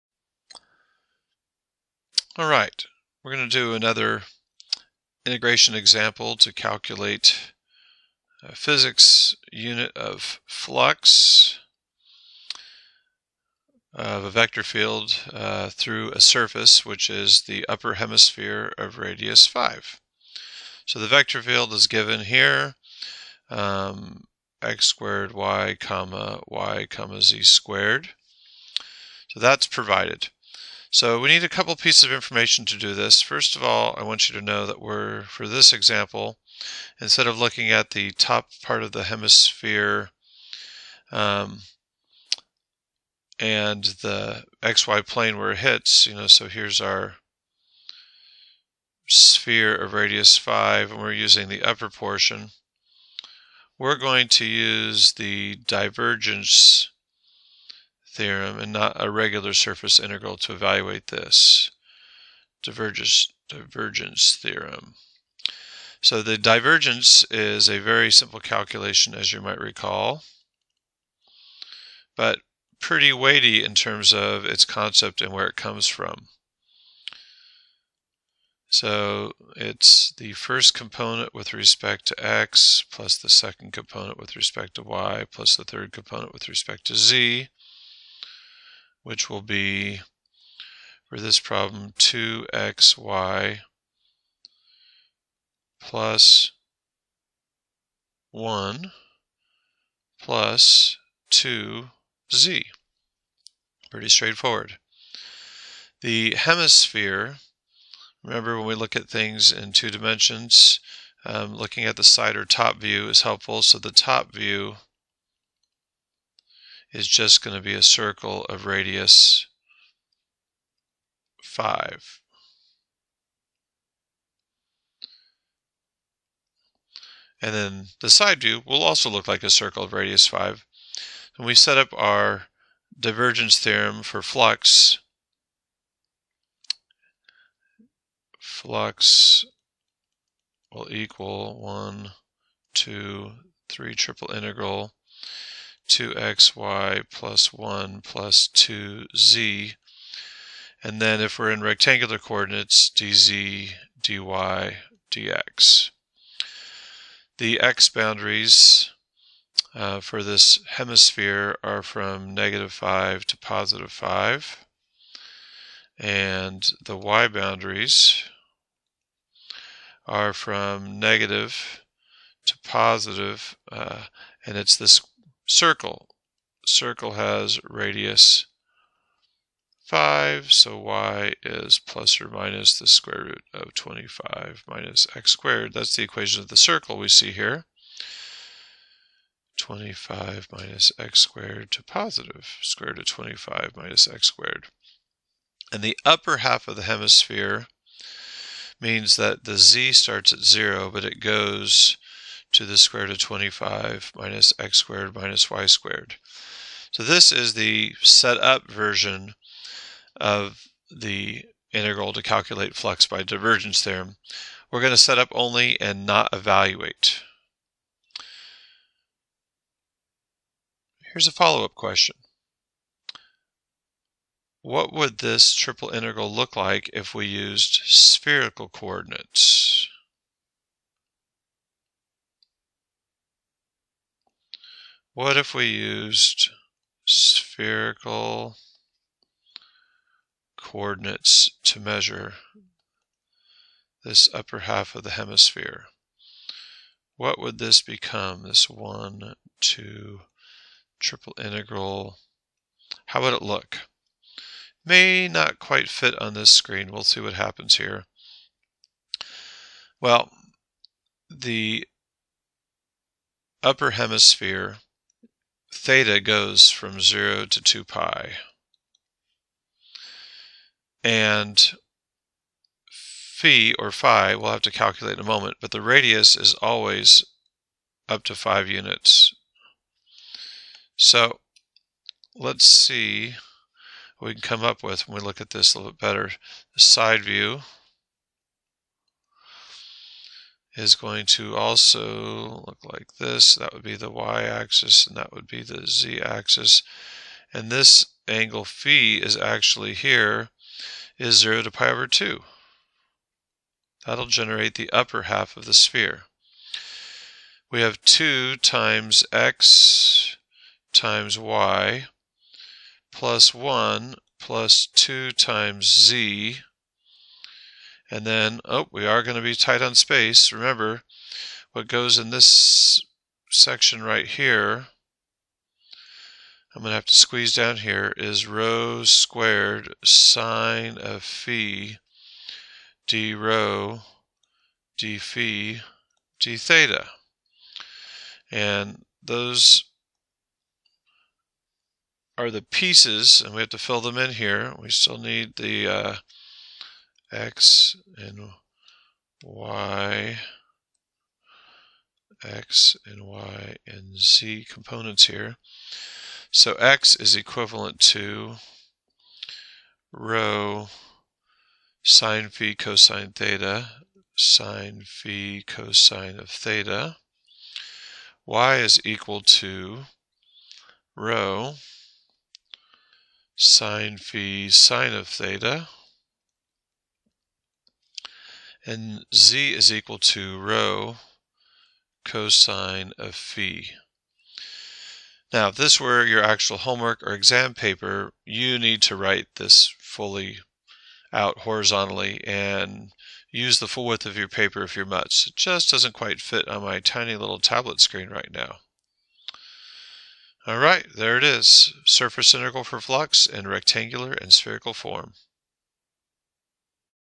Divergence Theorem Ex. 1
Here is an example of setting up the Divergence Theorem for Flux over a surface.  The triple integral is set up both in rectangular and spherical form.  Remember, some aspects are left for the student to "fill in" based on our reading and classroom lectures.